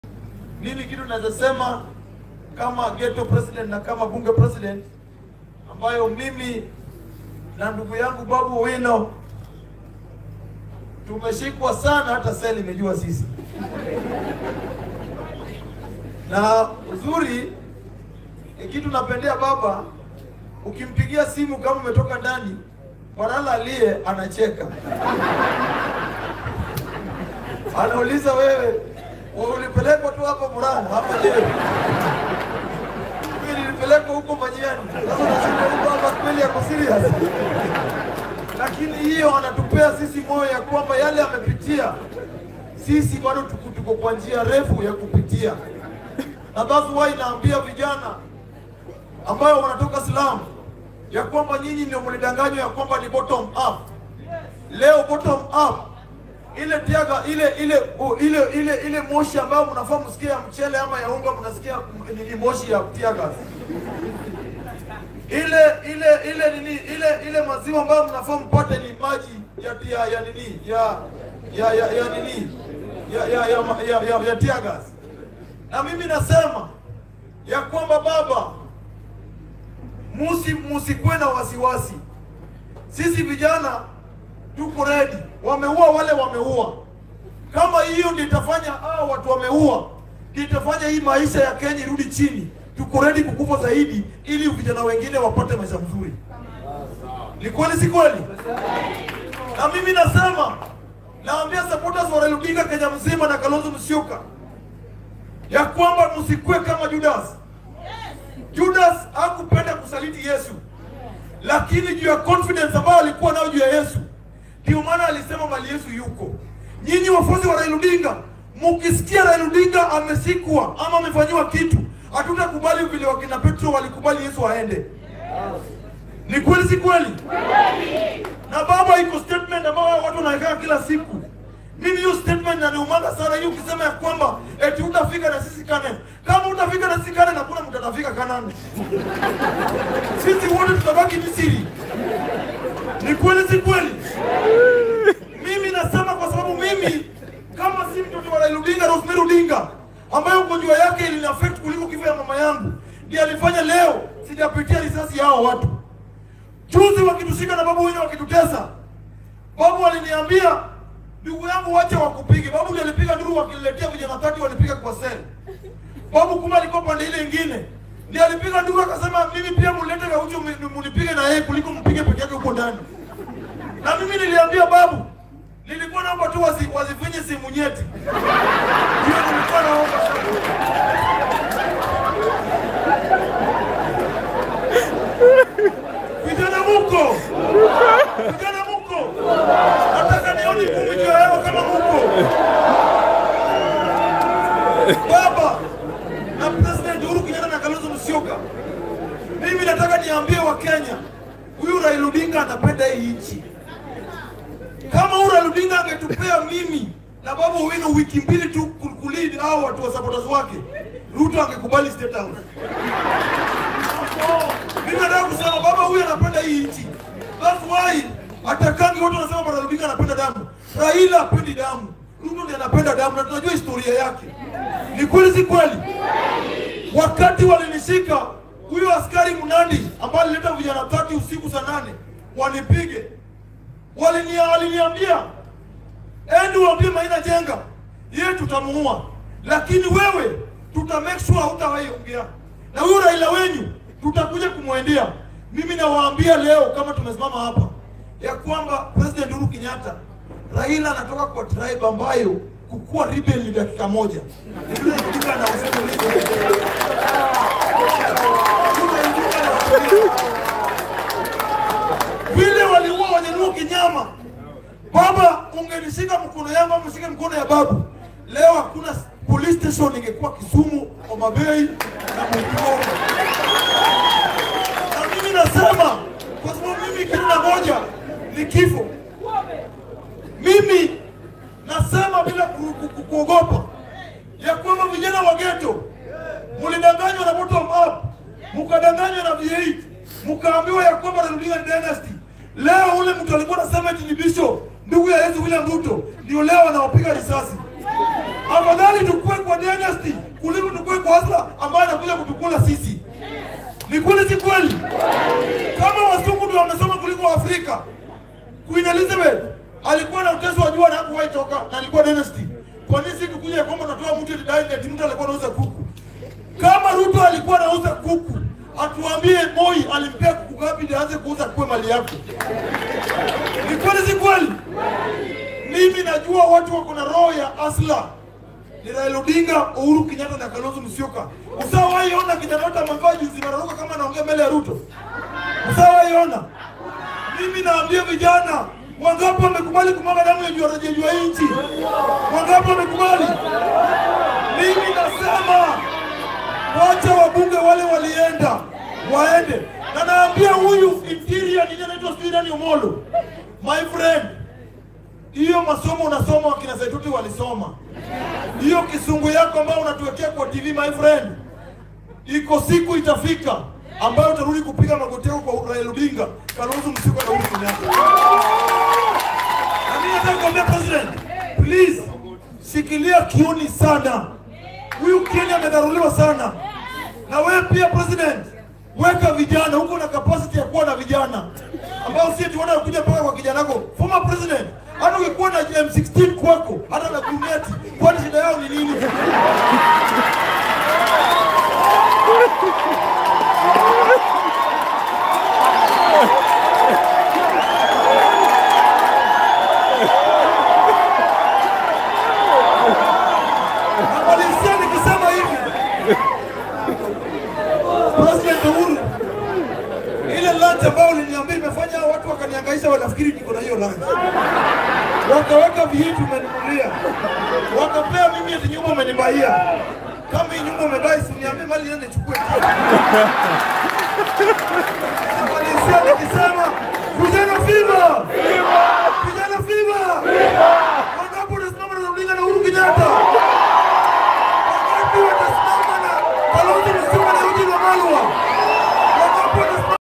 Gaucho risks arrest with his FLAMING REMARKS at Azimio requeim mass at Karen.
ACCURATE MEDIA KENYA is Kenya's number one online news channel Exclusively dealing with Politics from across the country.

Video made by ACCURATE MEDIA KENYA.